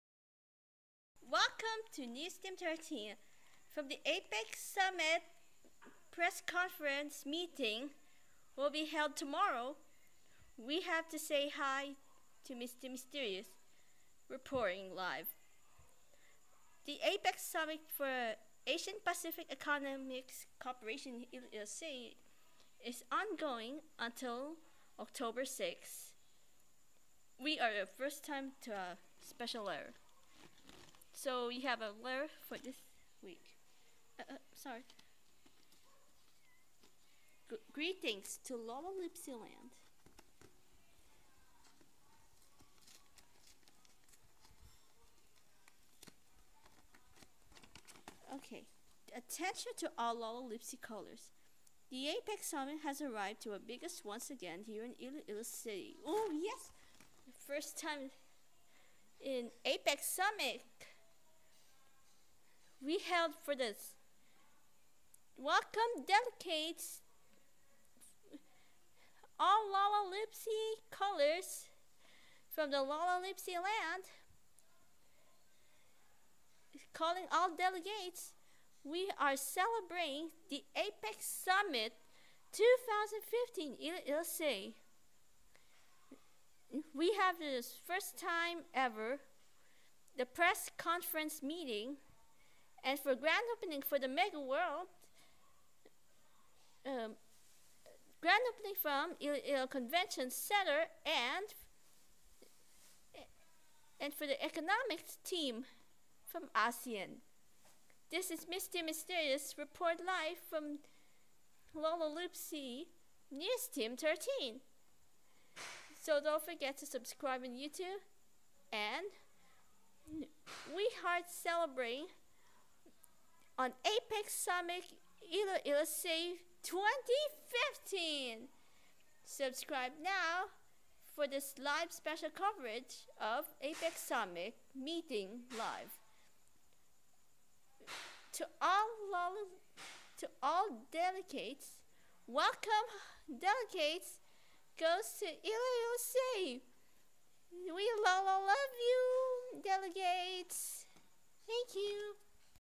Lalaloopsy News team 13: APEC Summit special prt 1
Misty reports on APEC Summit special update 2015.

The meeting is ongoing until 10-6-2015.
Spread the Lalalove in APEC!